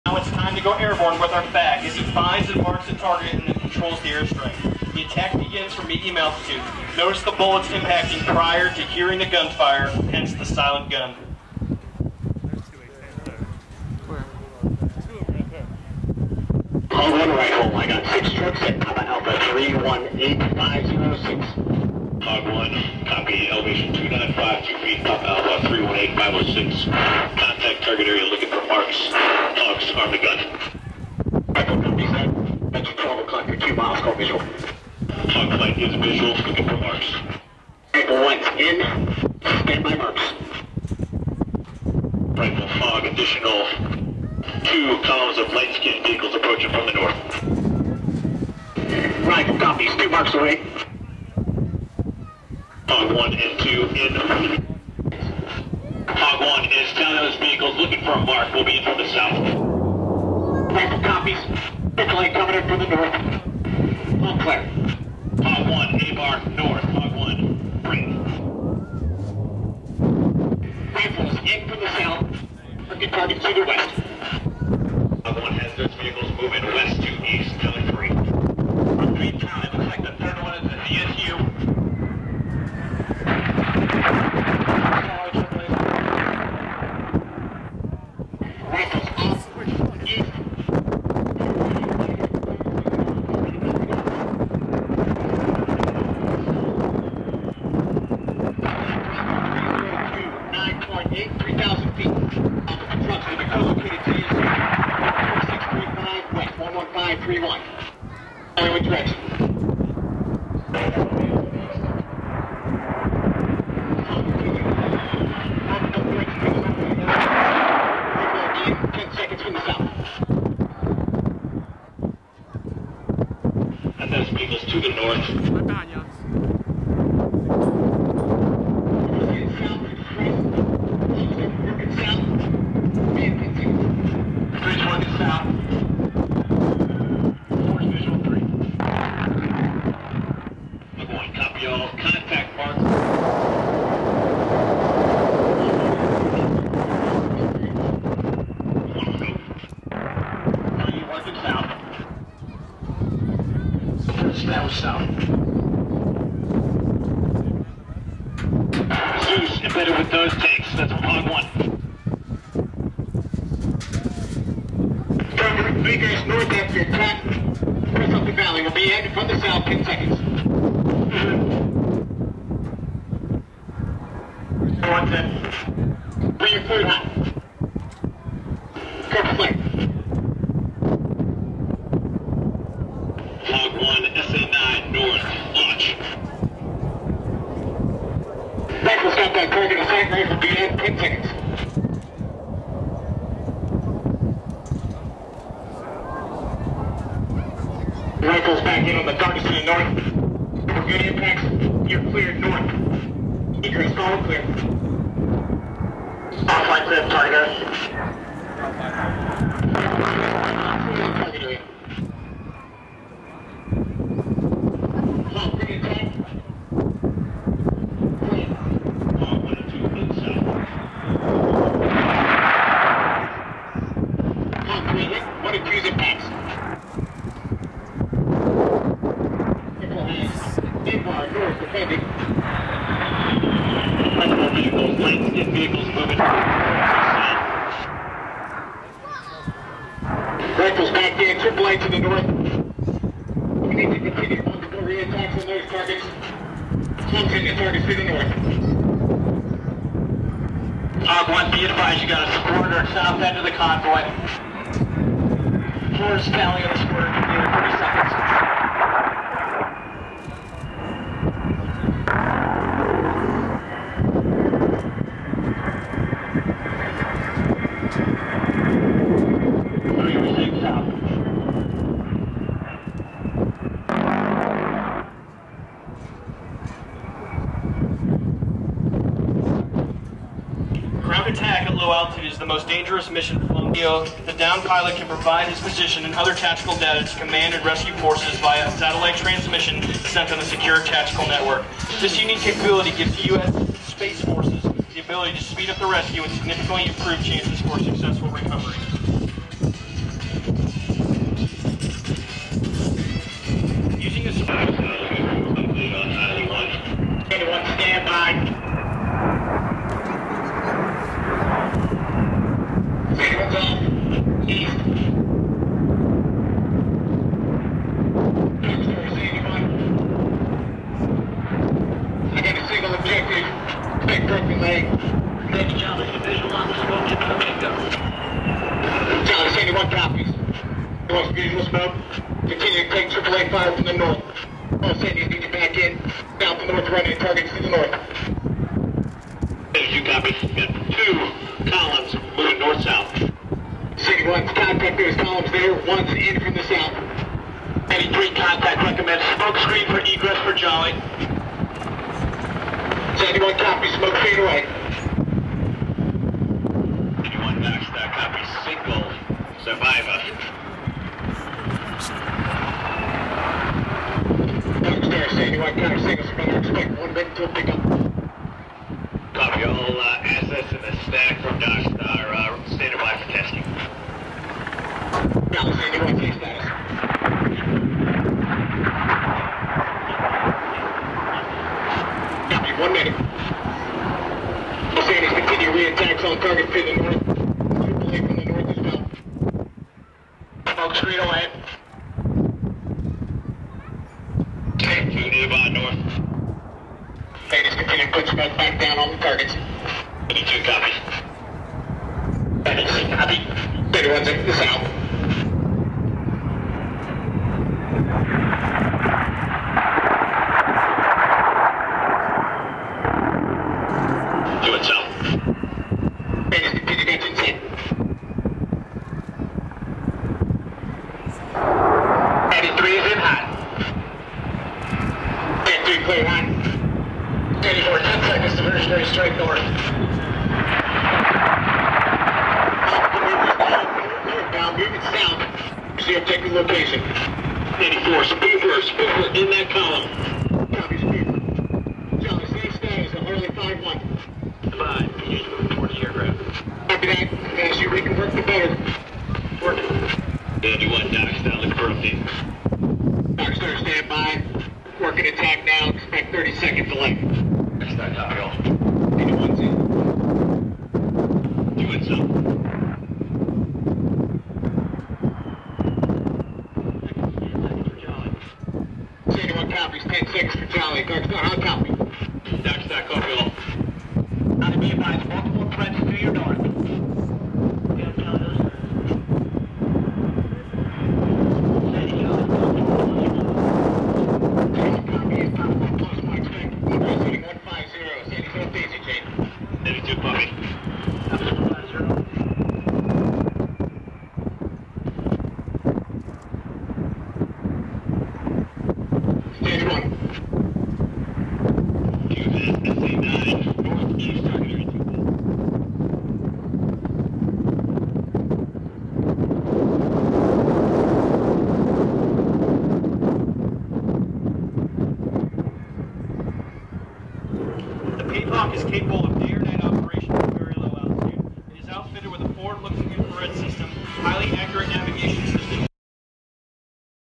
A-10 Thunderbolt Firepower (Capstone) Demo
A-10 Firepower Demo and downed pilot rescue